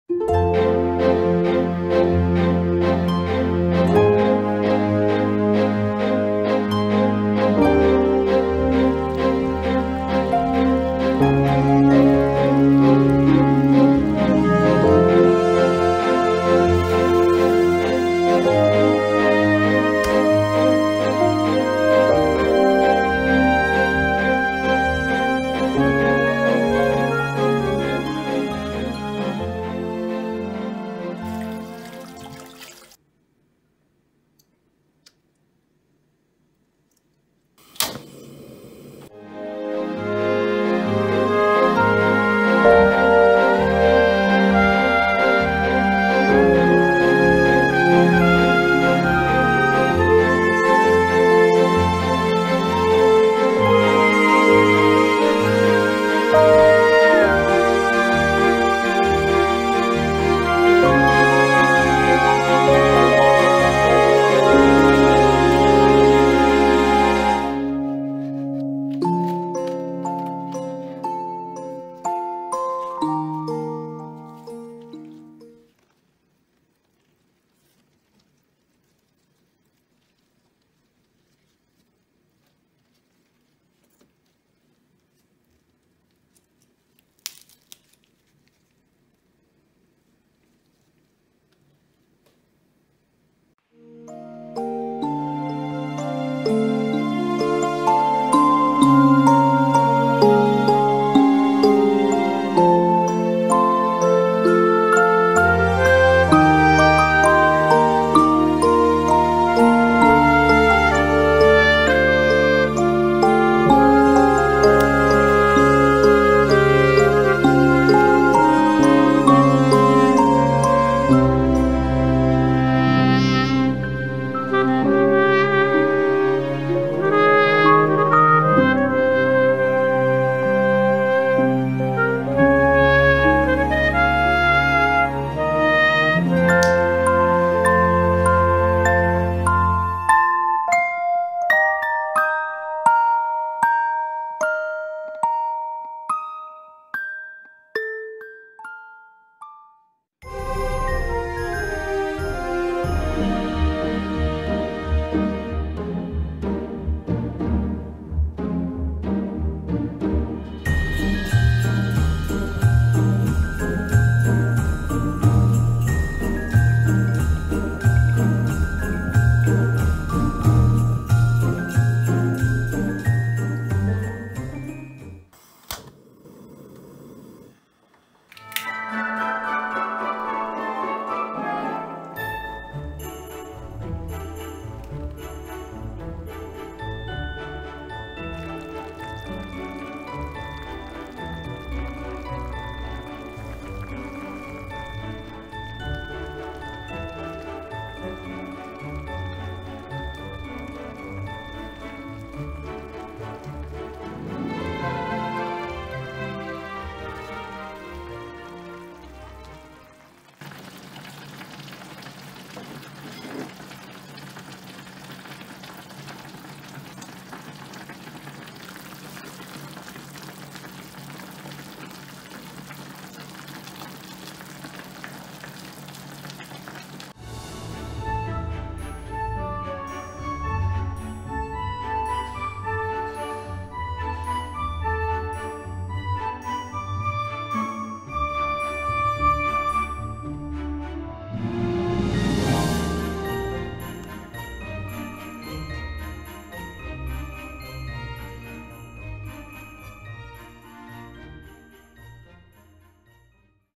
어묵 썰매 타고 나베 섬에 온 산타🎅 크리스마스 요리 (feat. 밀푀유나베) Christmas cooking
저마다의 크리스마스가 빛나는 연말이에요.
크리스마스를 더 따뜻하게 만들어 줄 식탁과 함께
소중한 사람들과의 지금을 즐기고
아름다운 연말, 아름다웠던 한 해로 기억되길 바라요 :D

[크리스마스 밀푀유나베 레시피]
-주재료: 알배추, 깻잎, 샤브용 소고기, 청경채, 버섯,
-데코: 소세지, 어묵, 게맛살, 김, 치즈, 파스타면, 케첩

-육수: 물130ml, 무, 다시팩, 표고버섯, 간장2, 쯔유1
-소스: 물2, 간장2, 식초1, 올리고당1, 설탕1

(쿠팡 파트너스 활동의 일환으로, 수수료는 영상 제작에 큰 힘이 됩니다 👐)